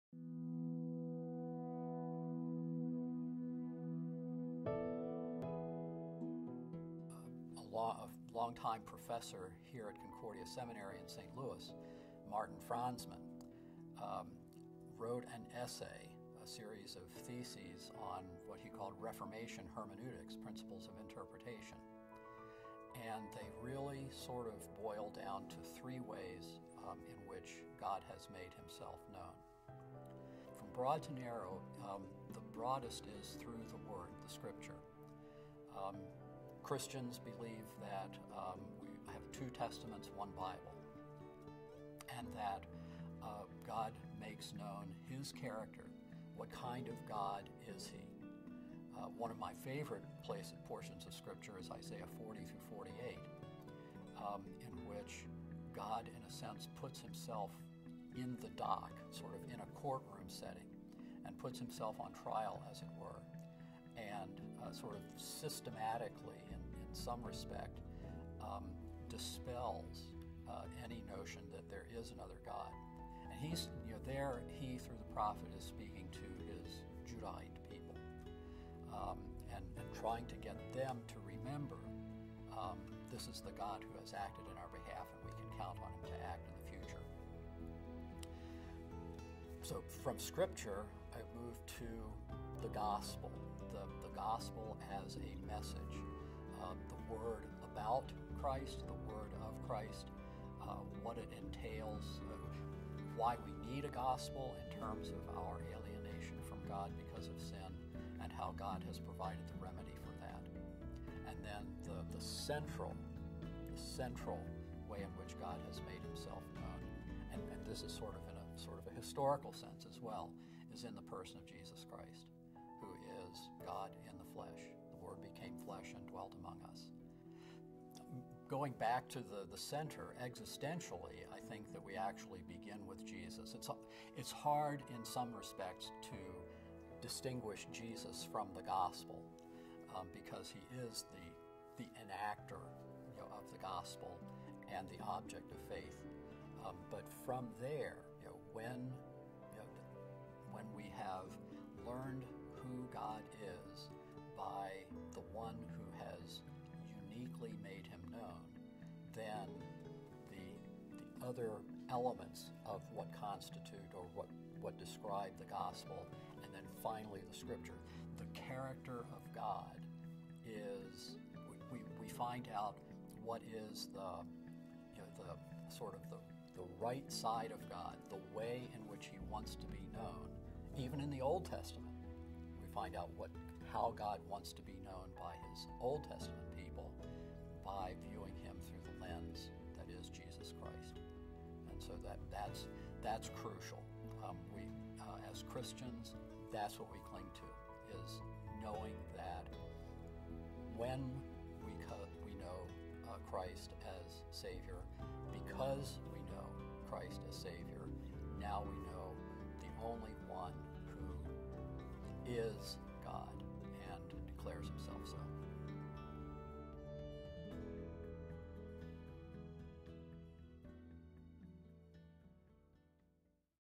Ways to Know God
How does God make himself known through special revelation?
Rev. William W. Carr

I have appreciated in recent years the writing of a college professor of philosophy. His last name is Budziszewski, and he wrote a book about natural law and the notion that it is sort of inscribed in us that there is a God, but that we cannot know him unless he makes himself known. And a long time professor here at Concordia Seminary in St. Louis, Martin Franzmann, wrote an essay, a series of theses on what he called Reformation hermeneutics, principles of interpretation, and they really sort of boiled down to three ways in which God has made himself known. One is and I'll work from sort of the broad to the narrow and see whether I can move back out. From broad to narrow, the broadest is through the Word, the Scripture. Christians believe that we have two Testaments, one Bible, and that God makes known his character what kind of God is he? One of my favorite portions of Scripture is Isaiah 4048 in which God, in a sense, puts himself in the dock, sort of in a courtroom setting and puts himself on trial, as it were, and sort of systematically, in some respect, dispels any notion that there is another God. And he's, you know, there, he, through the prophet is speaking his Judahite people and trying to get them to remember this is the God who has acted on our behalf, and we can count on him to act in the future. So, from Scripture I move to the gospel. The gospel has a message: the word about Christ, the word of Christ, what it entails, why we need a gospel in terms of our alienation from God because of sin, and how God has provided the remedy for that. And then the central way in which God has made himself known, and this is sort in a historical sense as well, is in the person of Jesus Christ, who is God in the flesh "The Word became flesh and dwelt among us." And, now, in a sense, sort of, I don't know, existentially or ontologically, it's knowing Jesus, knowing the gospel; those things come first because the Going back to the center, existentially, I think that we actually begin with Jesus. It's hard in some respects to distinguish Jesus from the gospel, because he is the enactor of the gospel and the object of faith. But from there, you know, when we have learned who God is by the one who has uniquely made him known, then the other elements of what constitute or what describe the gospel, and then finally the Scripture. The character of God is, we find out what is the, you know, the sort of the right side of God, the way in which he wants to be known. Even in the Old Testament, we find out how God wants to be known by his Old Testament people by viewing him through the lens that is Jesus Christ. And so that's crucial. As Christians that's what we clean to is knowing that when we know Christ of Savior, because we know God as Savior, now we know the only one who is God and declares himself so.